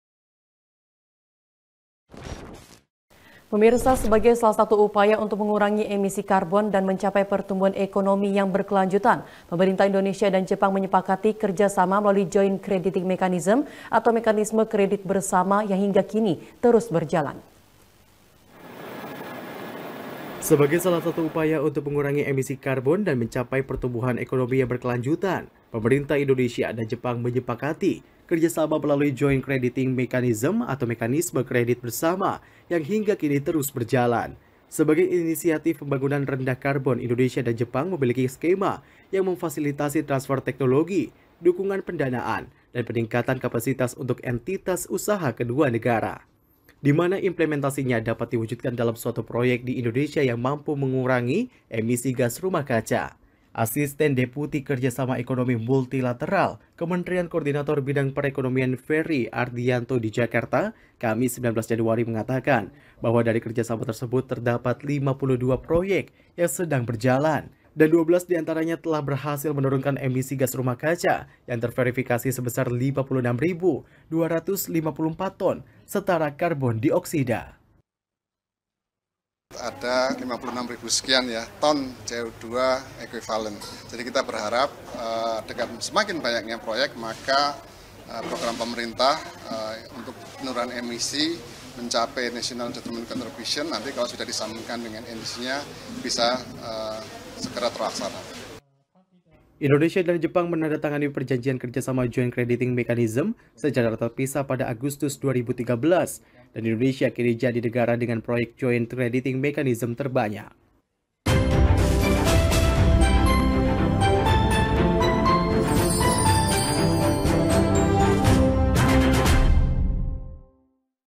Proyek Join Crediting Mechanism dalam Mengurangi Emisi Gas Rumah Kaca
Sebagai salah satu upaya untuk mengurangi emisi karbon dan mencapai pertumbuhan ekonomi yang berkelanjutan pemerintah indonesia dan jepang menyepakati kerja sama melalui ‘join crediting mechanism’ atau mekanisme kredit bersama yang hingga kini terus berjalan.